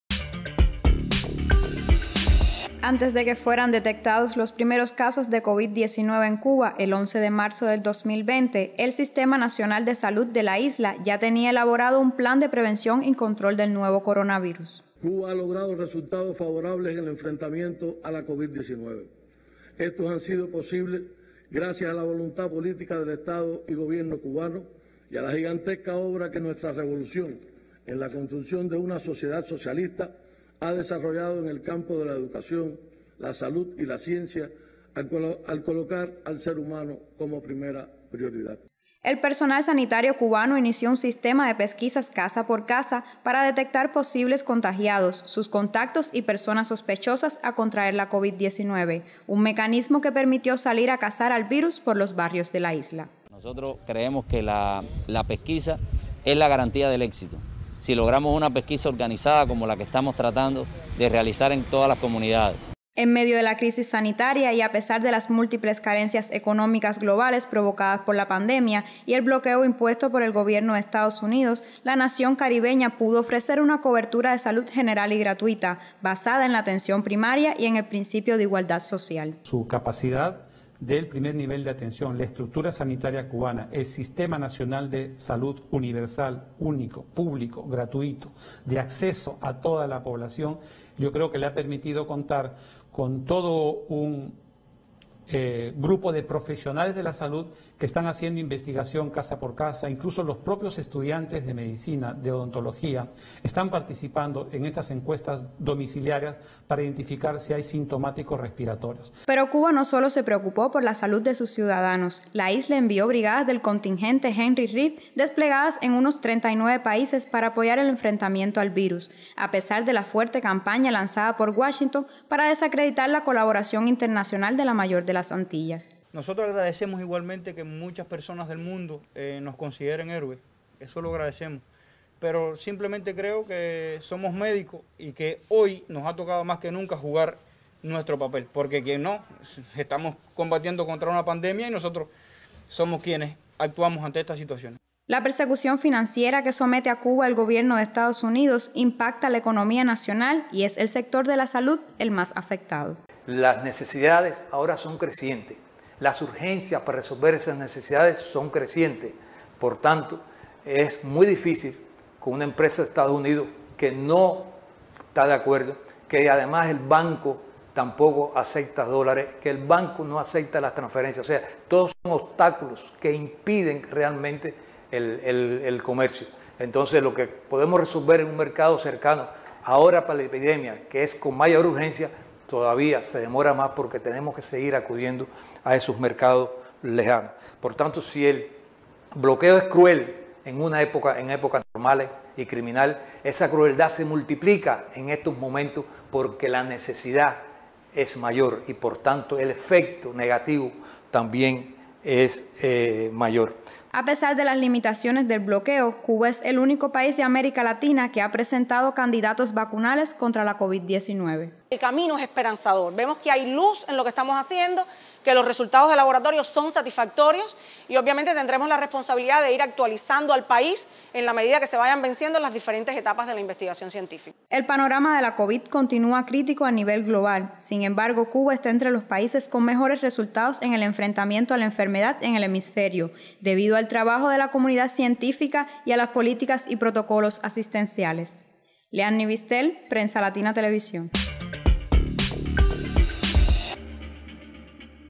Lo que realza al sistema de salud de Cuba en el enfrentamiento a la COVID-19
Cuba ofrece a sus ciudadanos una cobertura sanitaria universal y gratuita, basada en la igualdad social. (Tomado de Prensa Latina)